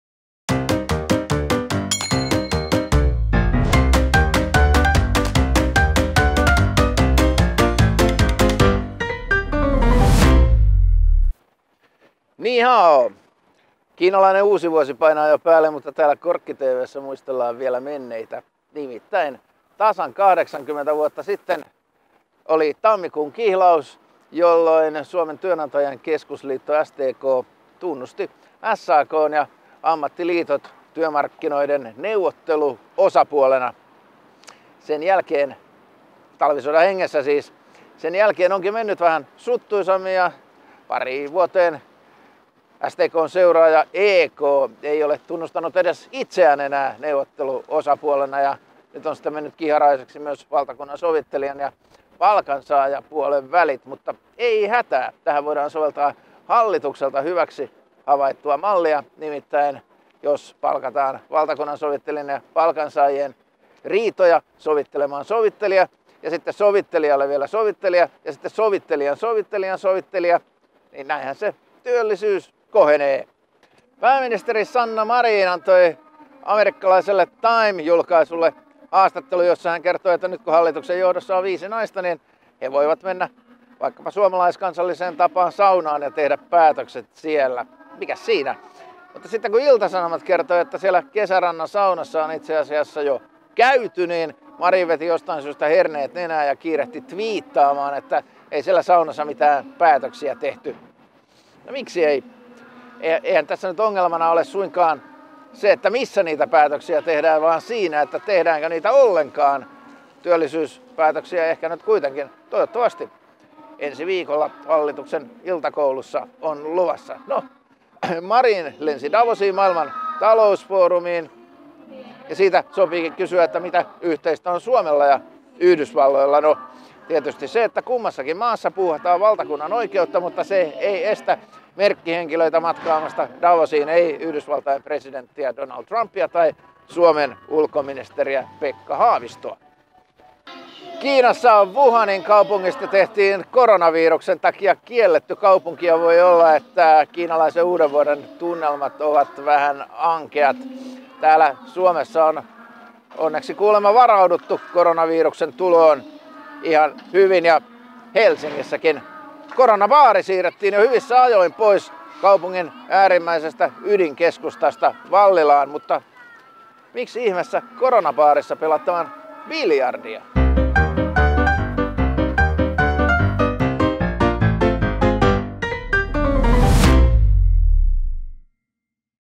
KorkkiTV: Sovittelijan sovittelijalle sovittelija
Hallituksen työllistämismallia voisi soveltaa myös palkansaajien ja valtakunnansovittelijan riitoihin.